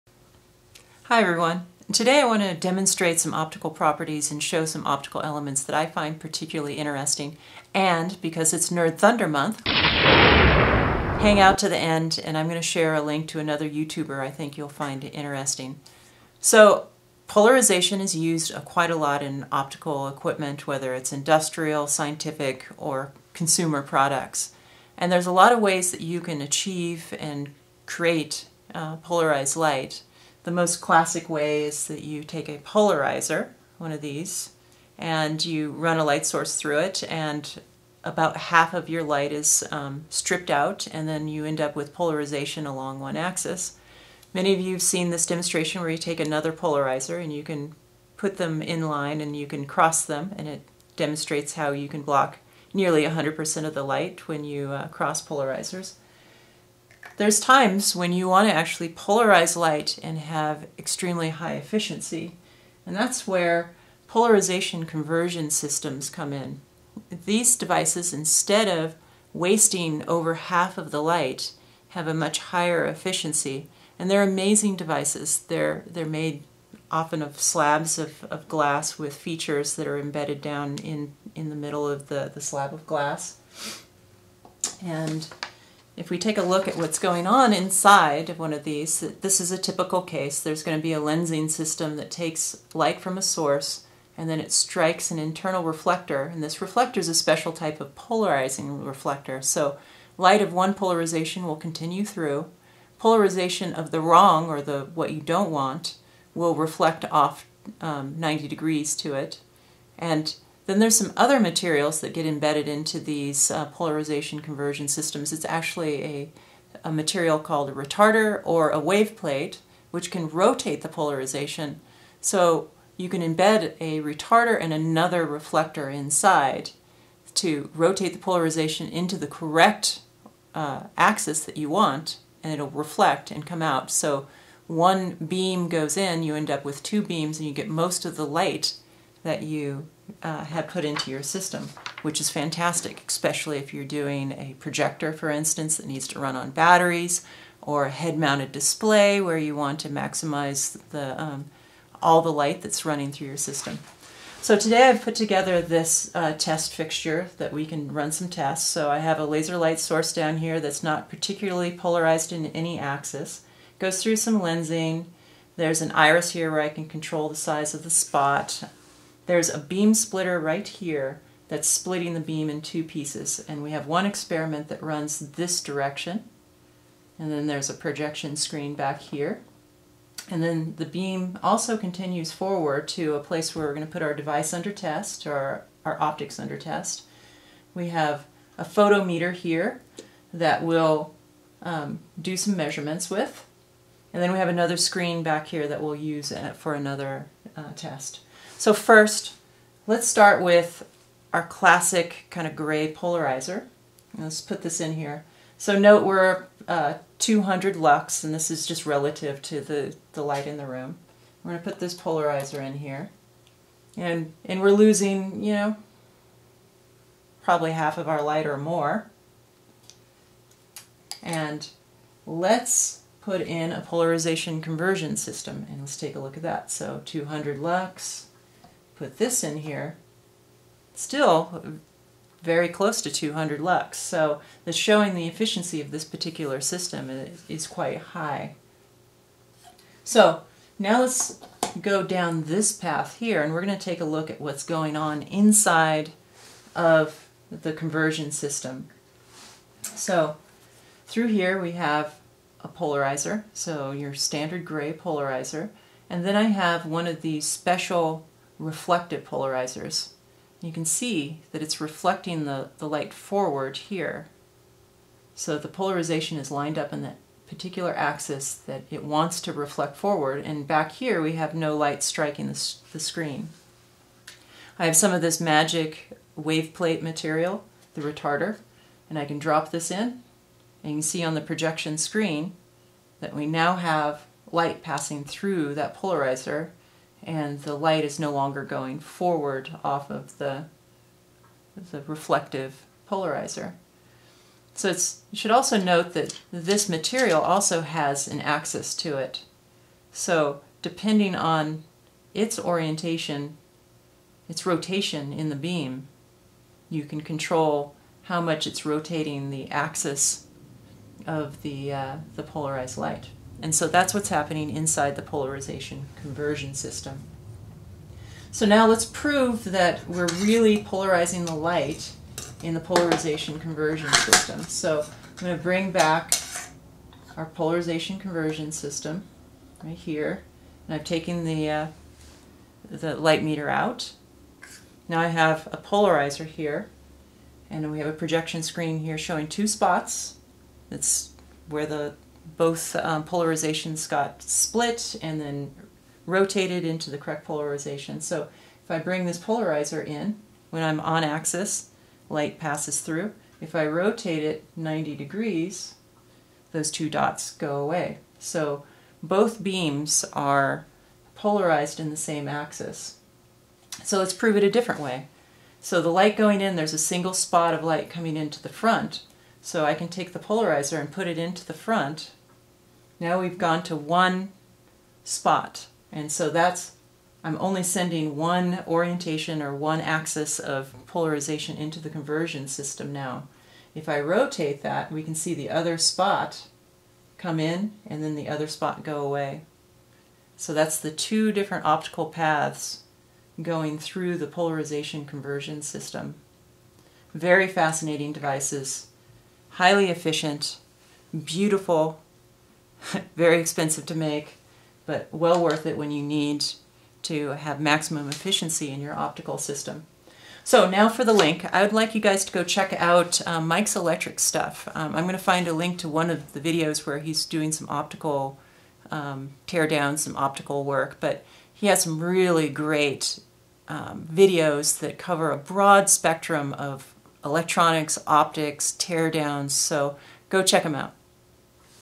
Polarization Conversion Systems
Demonstration of wave plates/retarders, polarization specific reflectors, and polarization conversion systems.